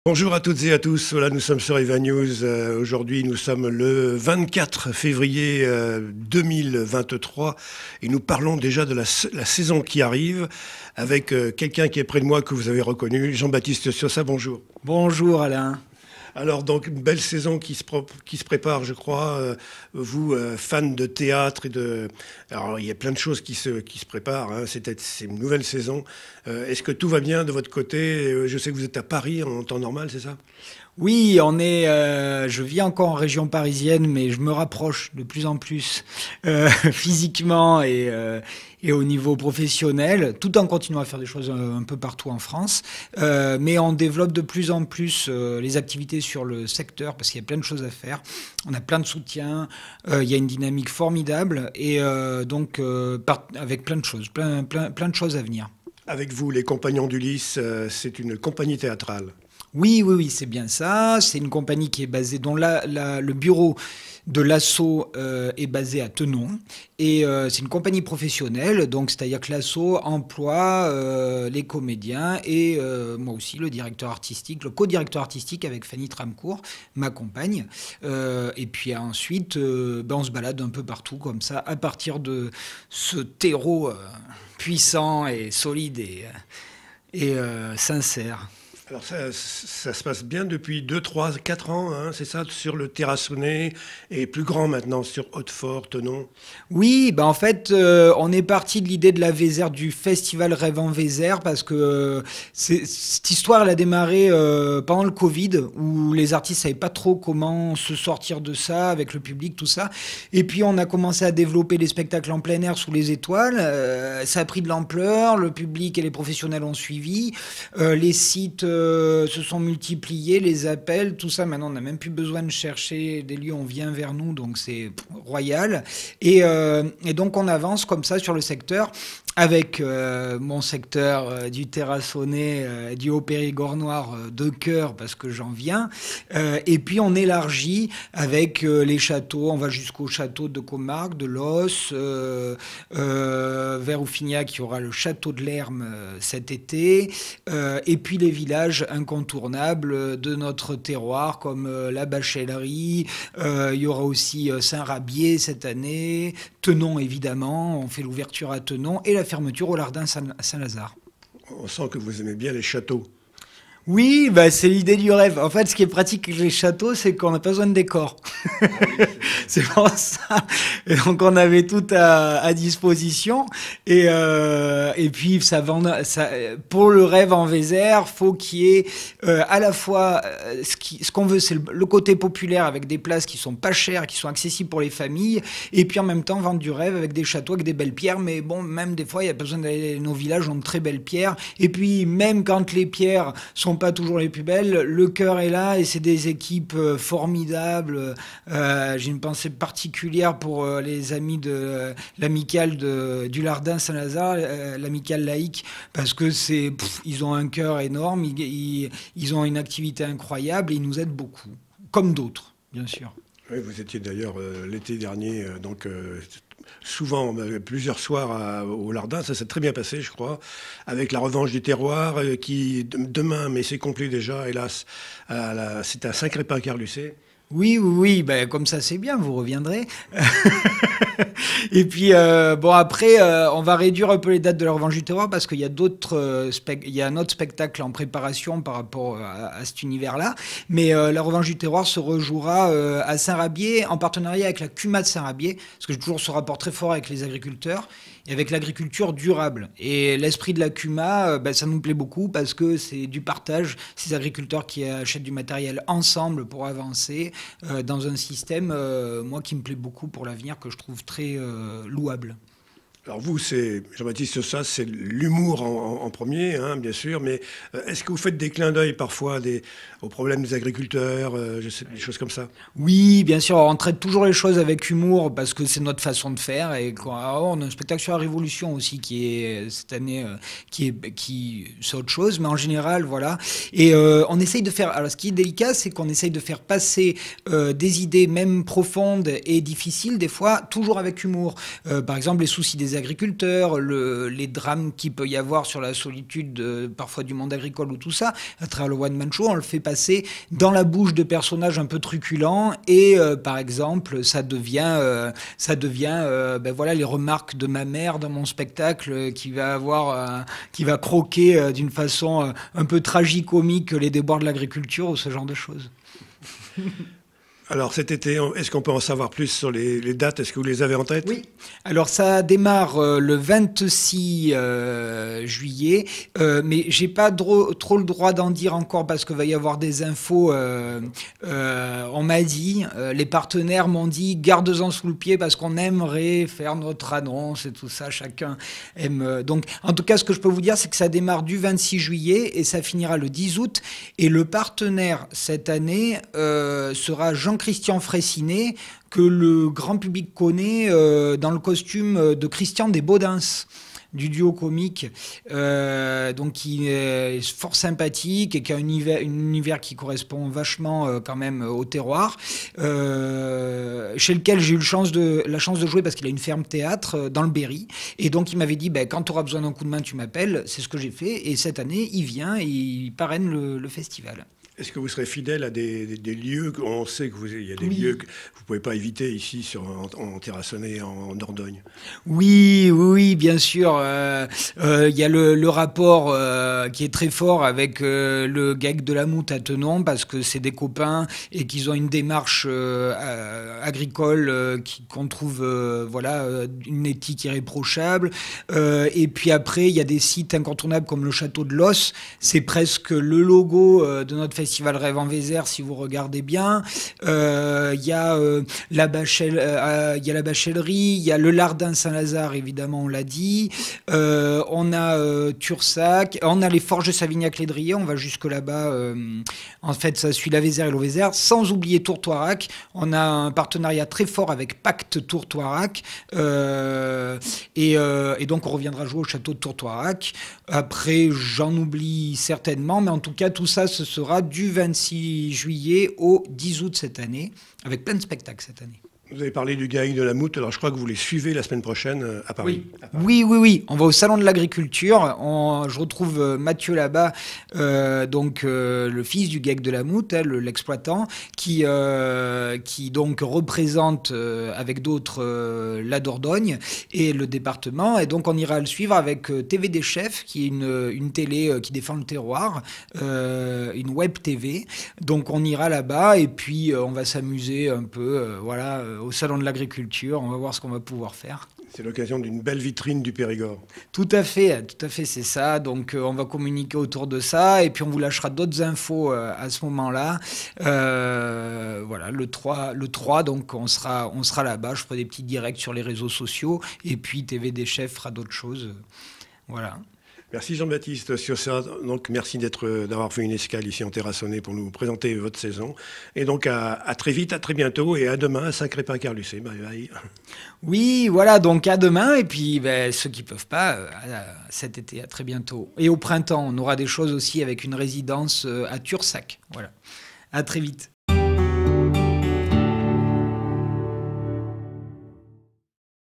Jean-Baptiste Siaussat interview fév. 2023
Le comédien est de retour en Dordogne cet été 2023. Interview pour Ewanews le vendredi 24 février 2023...